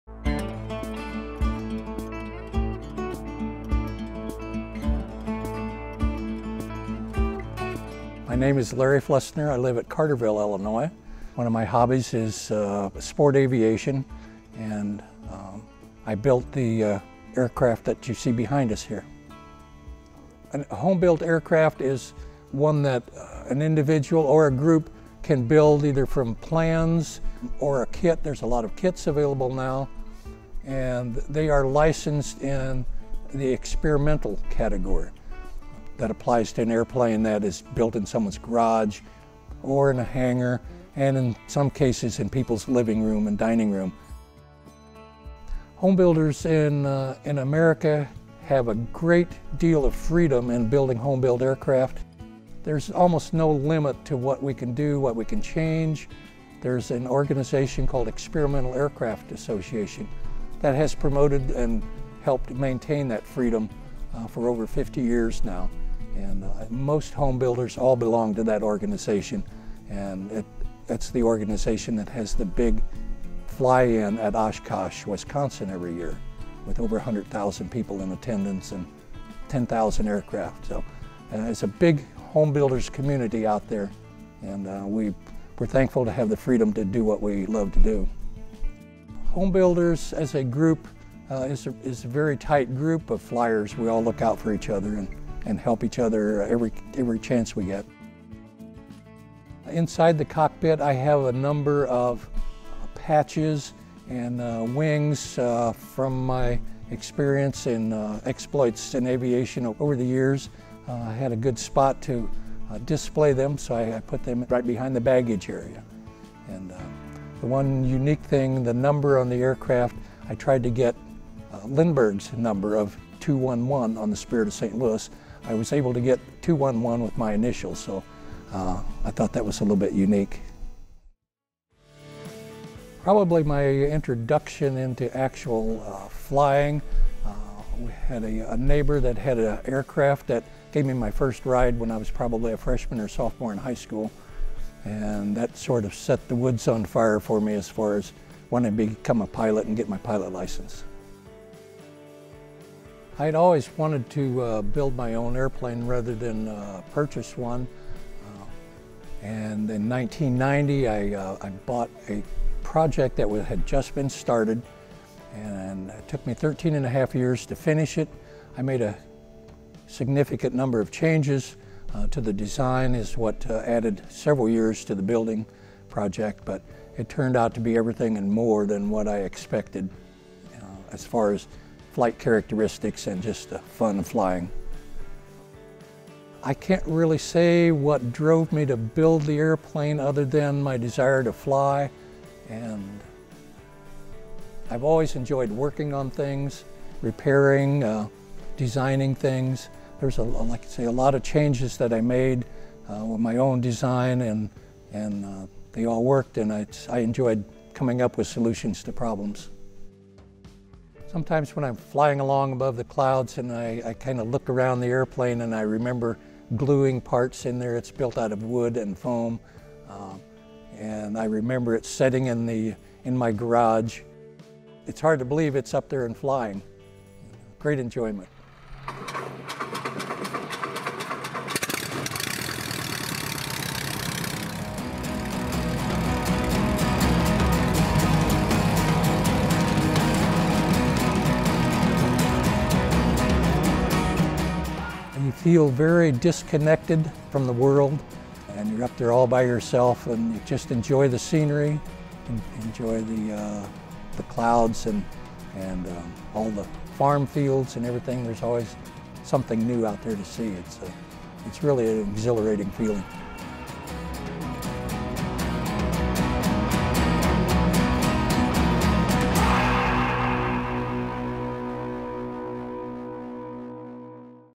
Homebuilt Airplane: From Glue in the Garage to Soaring Skies l alt.news 26:46
Homebuilt Airplanes are a piece of aviation history that greatly exhibit the liberty that engineers and aviators have. Larry Flesner is one of the many men who spend years in their garage, crafting and tooling to create their own aircraft. Step aside, Wright Brothers.

Produced:
Bryce Daly
Crew:
Hayley Wash
Ben Giles
Mikhail England
Jeremiah Teal